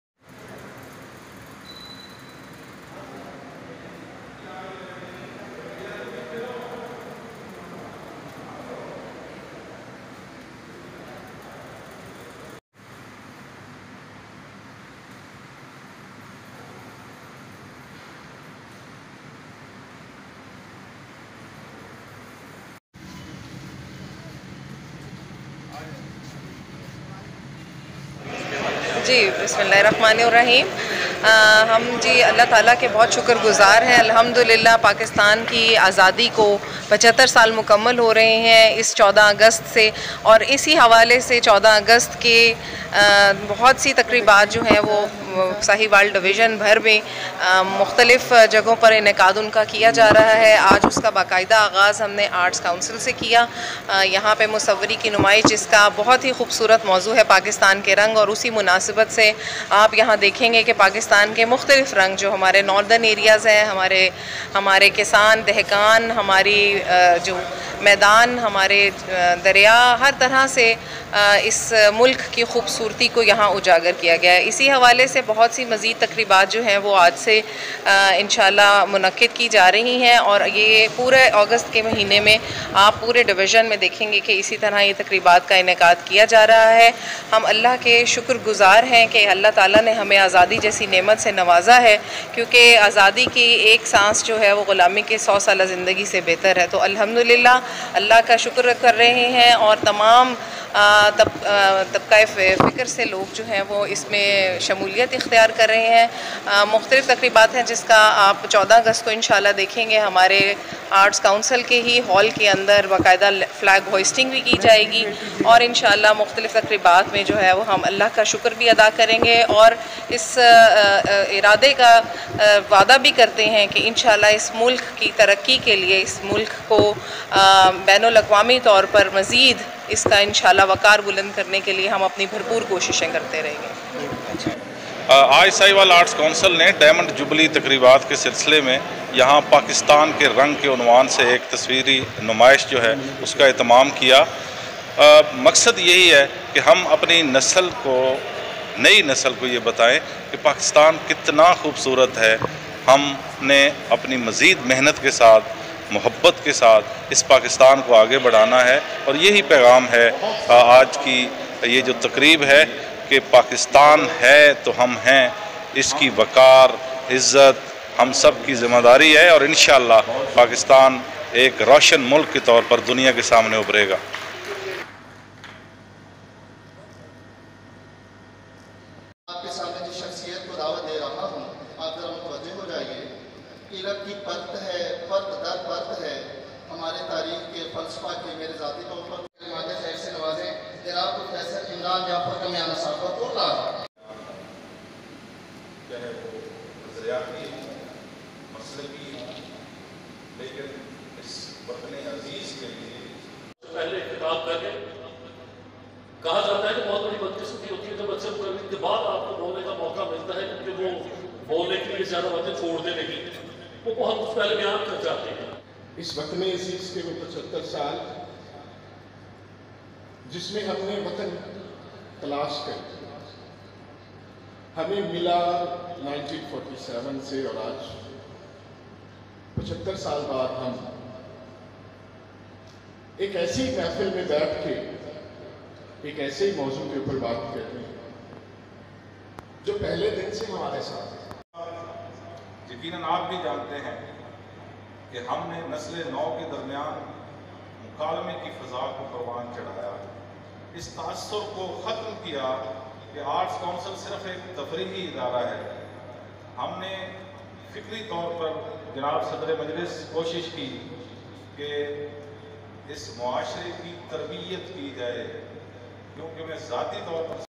Diamond Jubilee celebrations of Pakistan's Independence Day at Jinnah Hall Sahiwal PakistanTimelines
Photo exhibition and seminar at the Arts Council in connection with the Diamond Jubilee celebrations of Pakistan's Independence Day